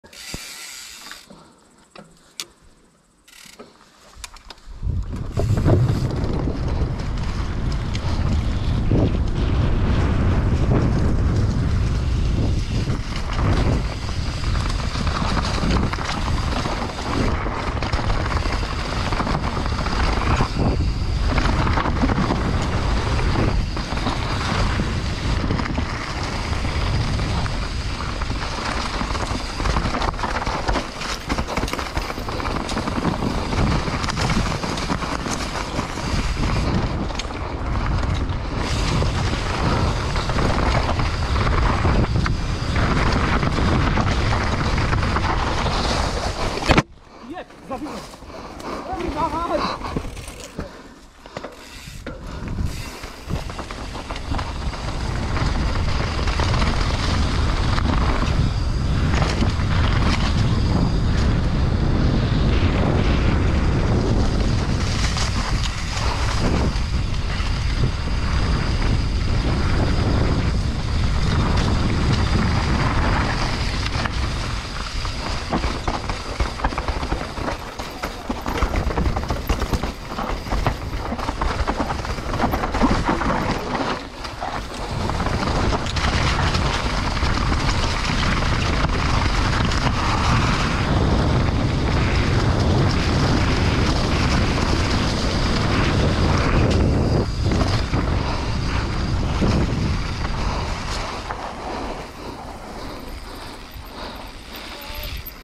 Downhill - Stary Groń Brenna 2021 POV
Przejazd z trasy downhill na Starym Groniu w Brennej 2021.

Wsparcie:
Black Bike - Serwis Rowerowy Rybnik